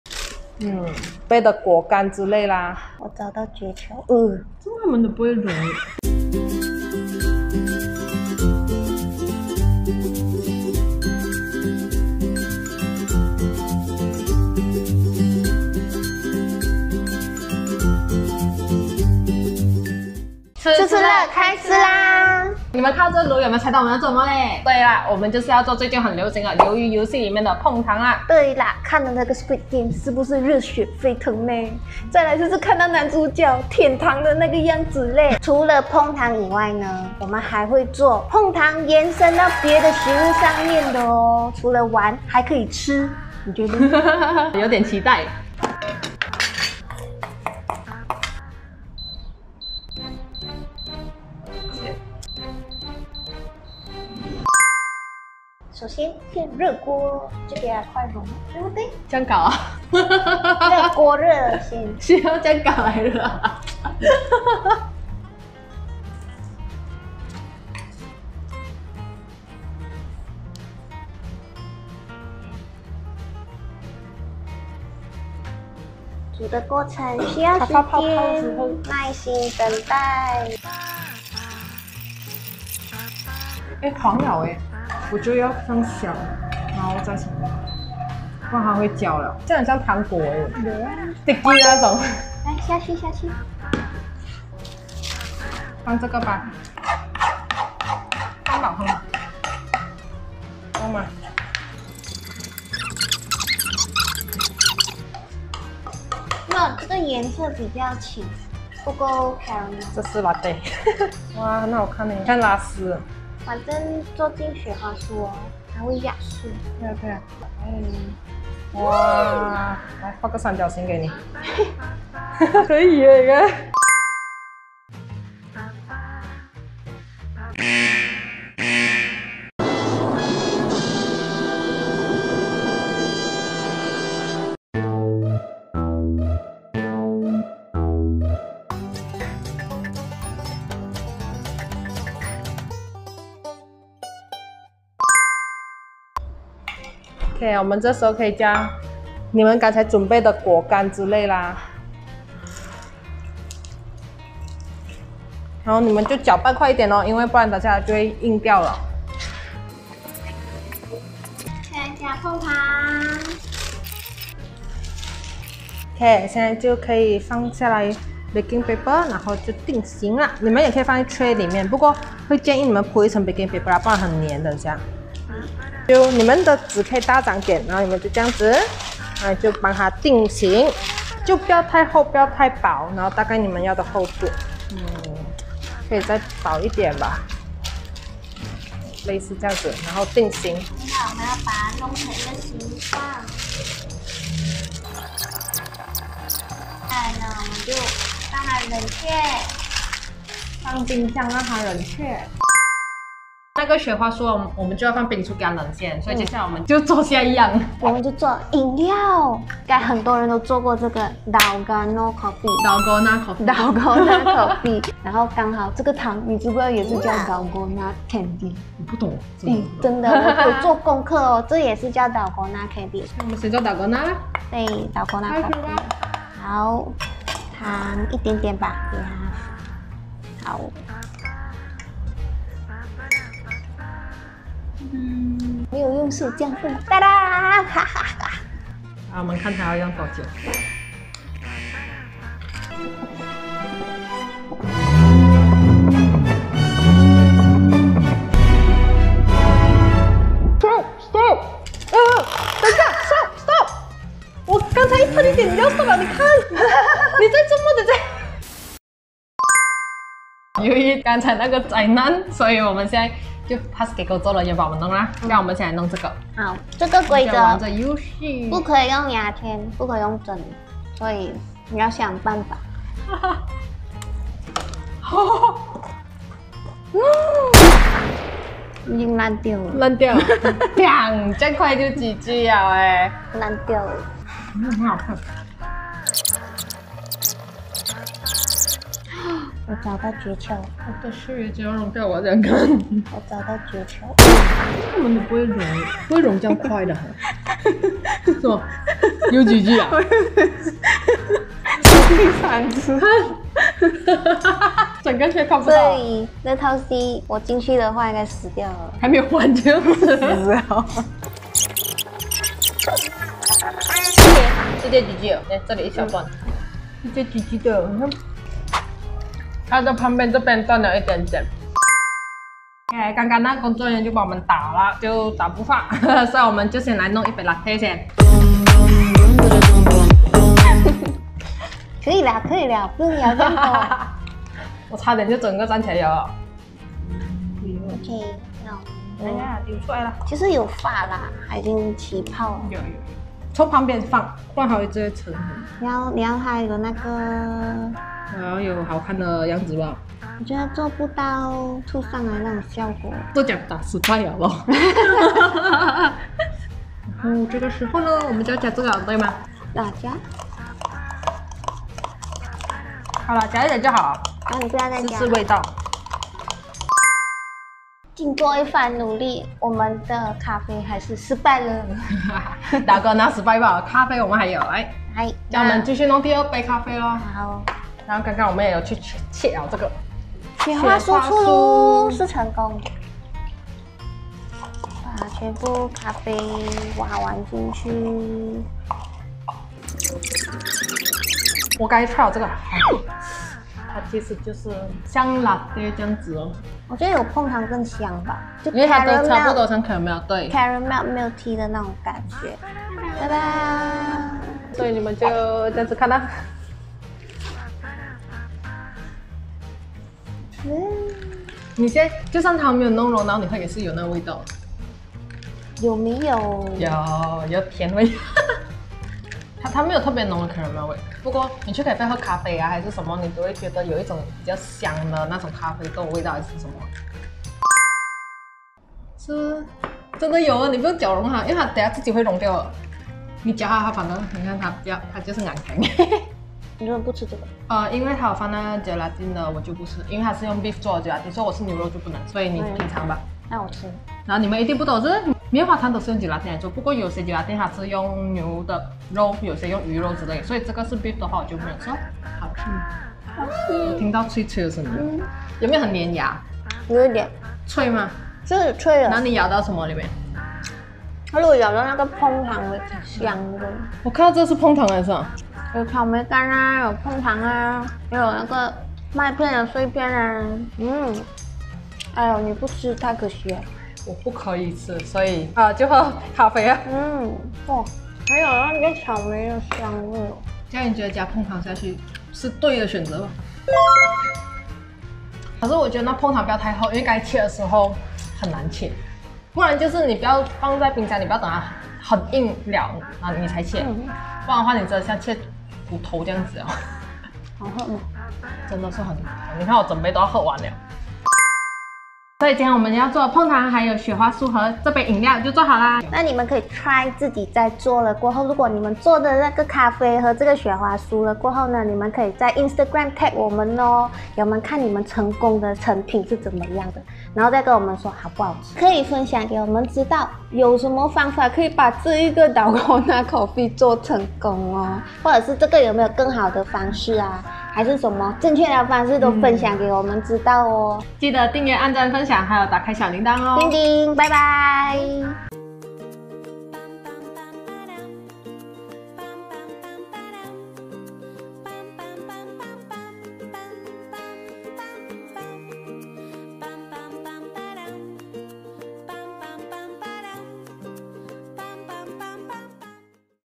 【美食探險】大翻车! 灾难现场! 我们竟然玩得比鱿鱼游戏更狼狈! 椪糖还能这样吃! 🐙The hits of Squid Game! Fusion with Dalgona Candy🐙
《魷魚遊戲》是於2021年9月17日全球上線的Netflix韓國原創劇，本劇由黃東赫執導和編劇，也是他首度執導Netflix原創劇。
該劇講述456名背景各異但一樣債台高築的玩家，為了贏得456億韓元的獎金參加一系列致命的兒童遊戲。該系列的名字取自於名字相似的韓國兒童游戲。黃東赫根據自己早年的經濟困難以及韓國的貧富差距，構思了該劇的事件。